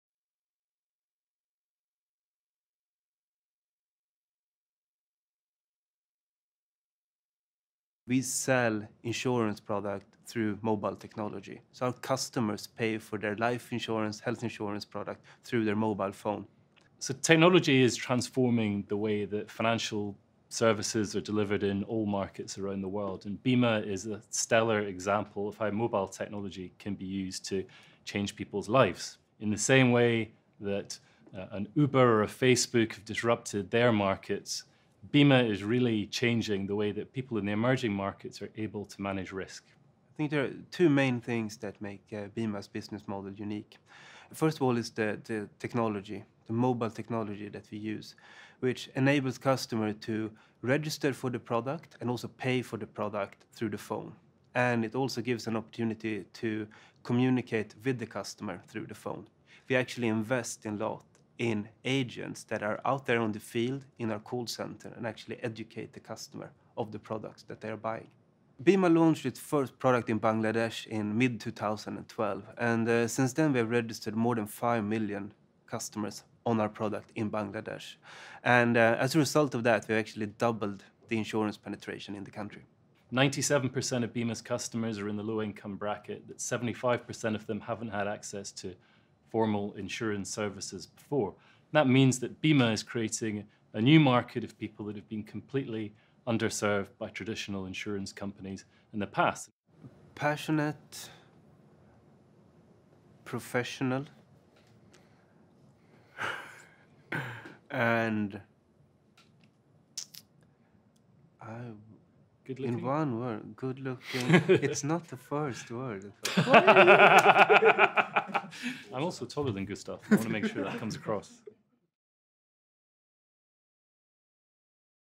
BIMA: Reaching the unreachable
BIMA CEO Gustaf Agartson and LeapFrog’s Stewart Langdon discuss how BIMA’s model is disrupting the traditional model of insurance distribution.